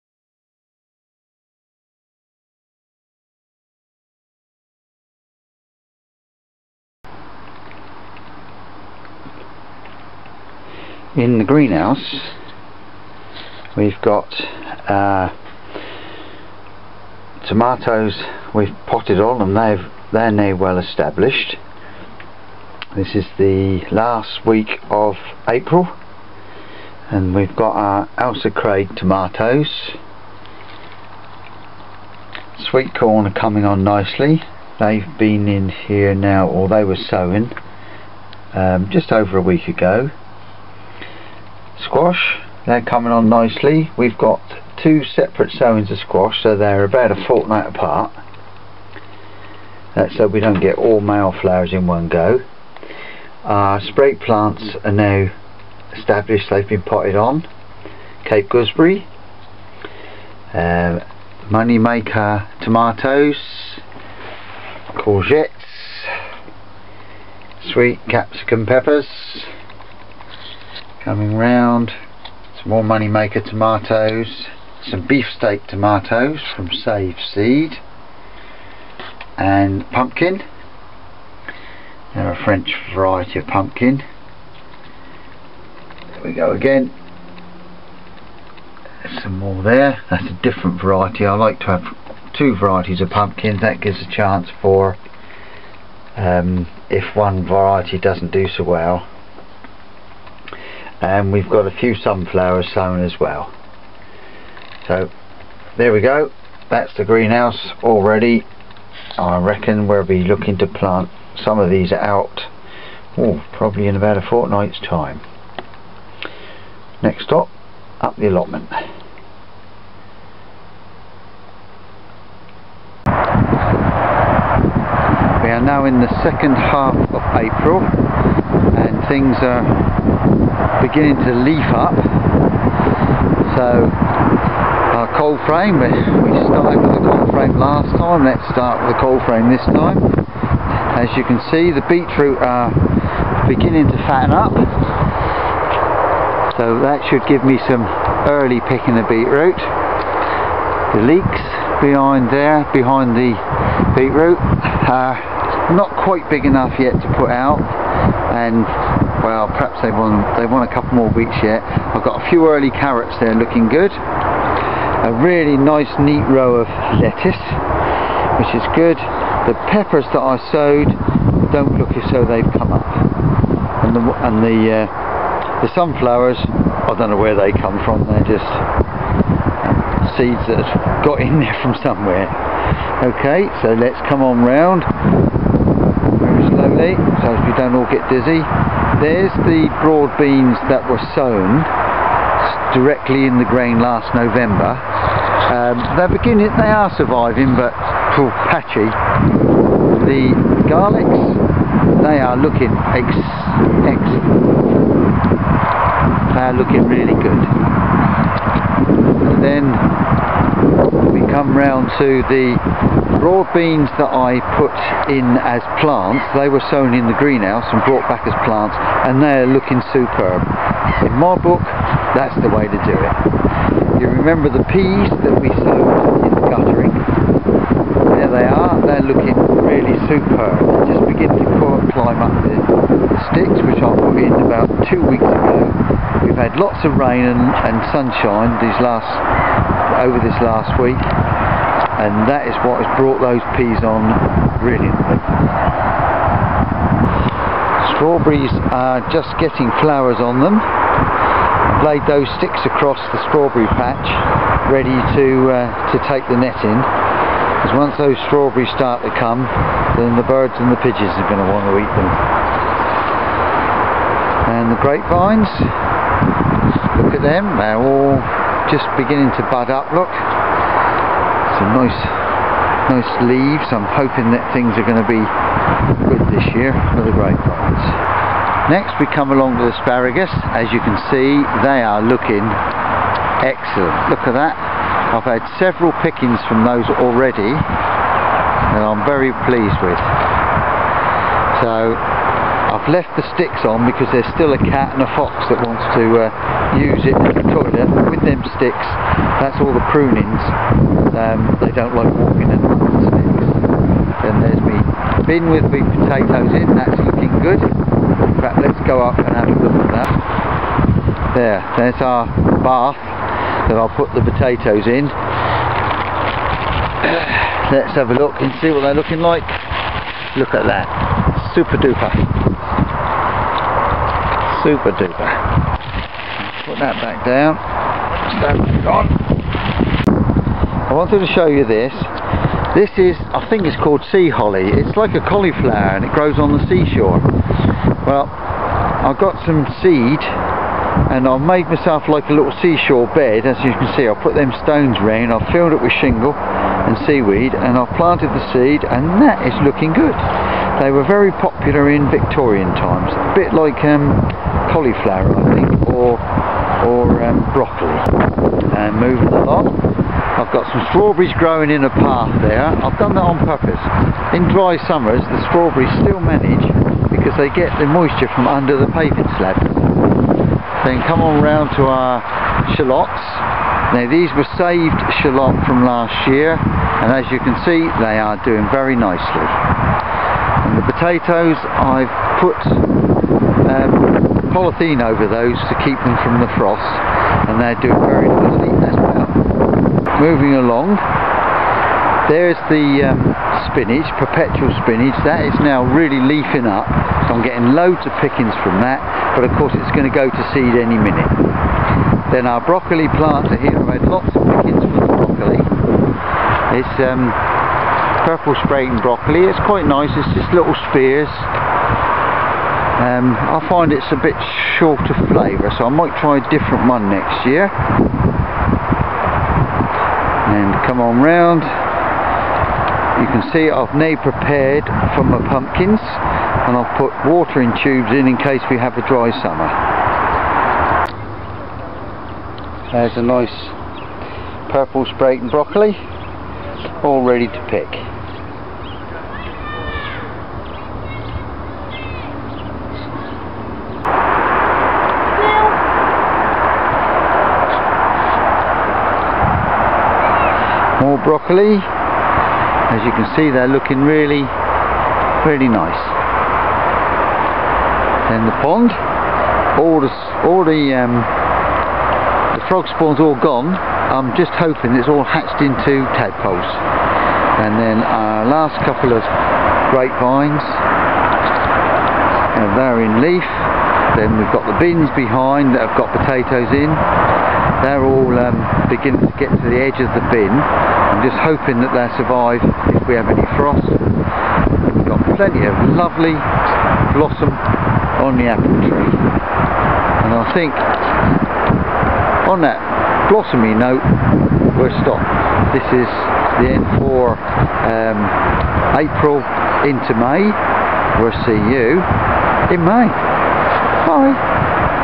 April to May on My vegetable plot.
April to May on my vegetable plot. How vegetables are progressing from last month and what is next to plant out.